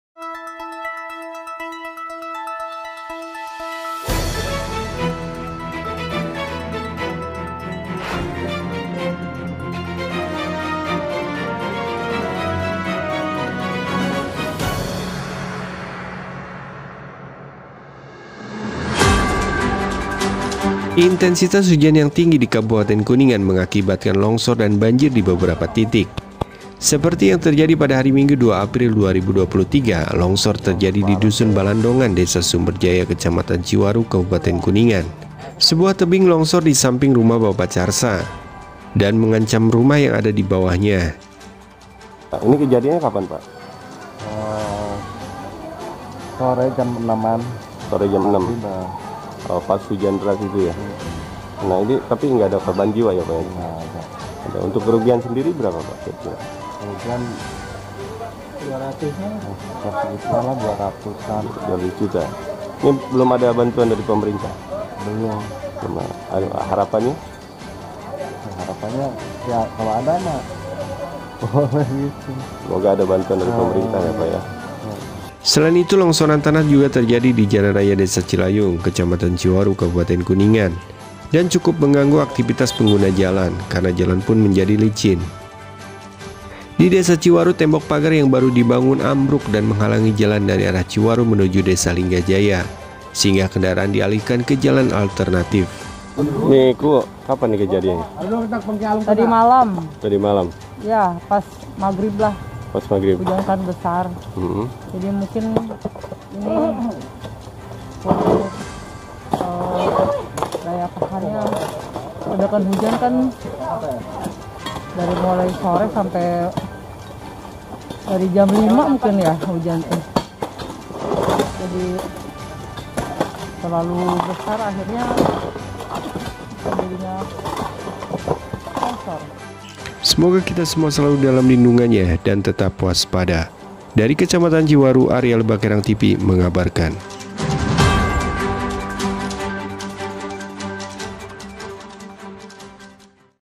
Tebing Longsor di Dusun Balandongan, Mengancam Rumah di Bawahnya, Kuningan Jawa Barat
Longsor di Kecamatan Ciwaru Kabupaten Kuningan Jawa Barat.